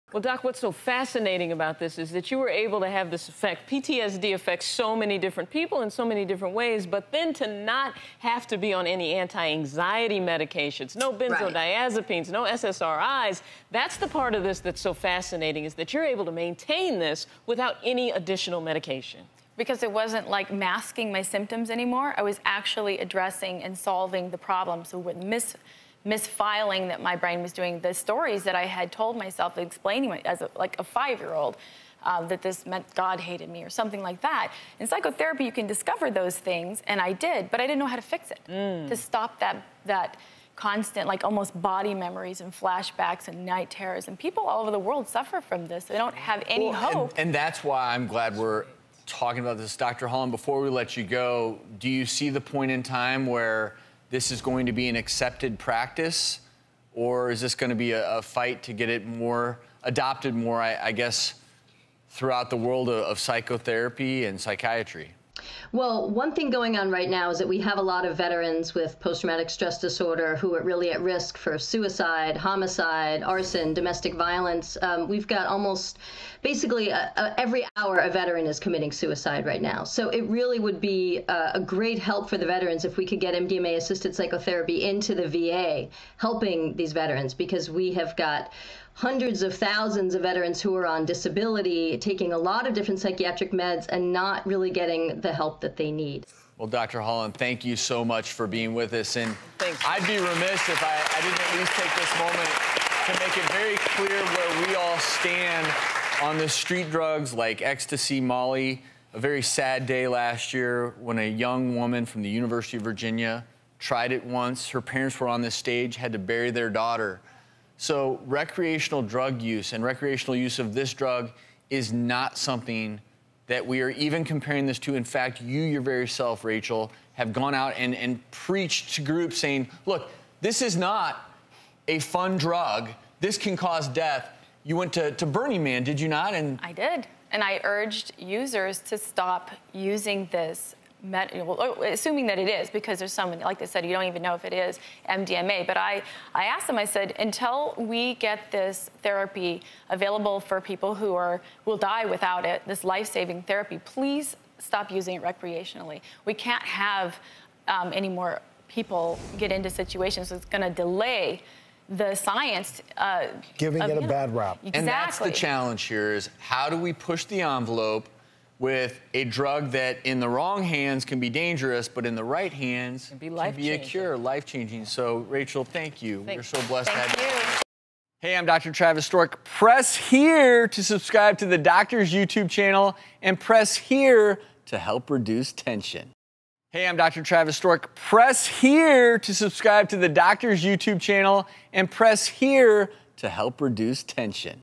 The Future of MDMA-Assisted Therapy
Psychiatrist and psychopharmacologist Dr. Julie Holland explains how this treatment could potentially be used to help war veterans suffering with PTSD.